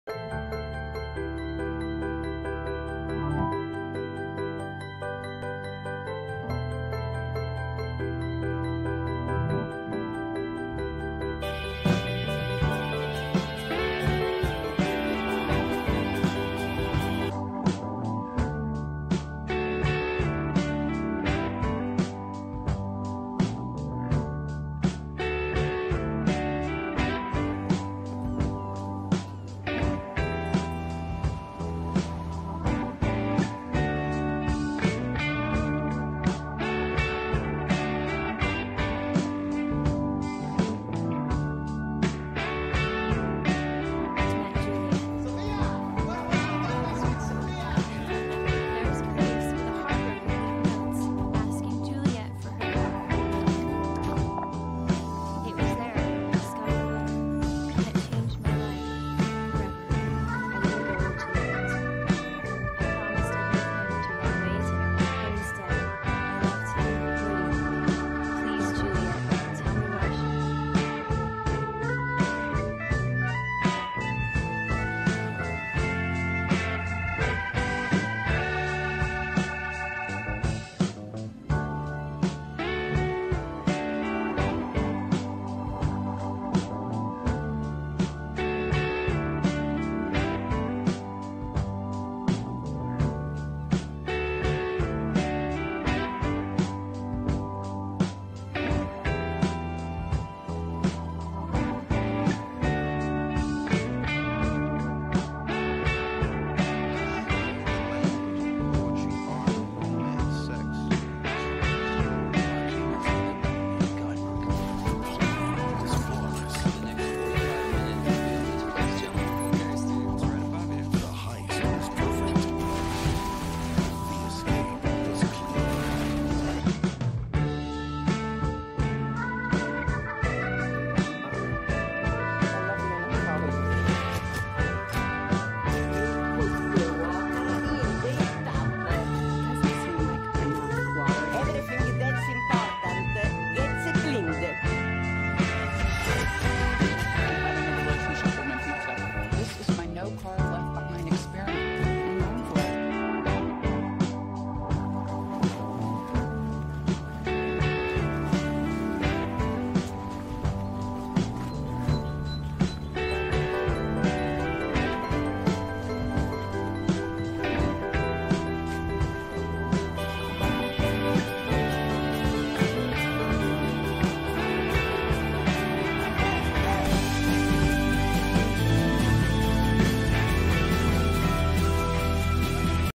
Five Great movies that will make you travel in Italy without leaving home - part 1
Italy is a great destination! Five Great movies that will make you travel without leaving home - part 1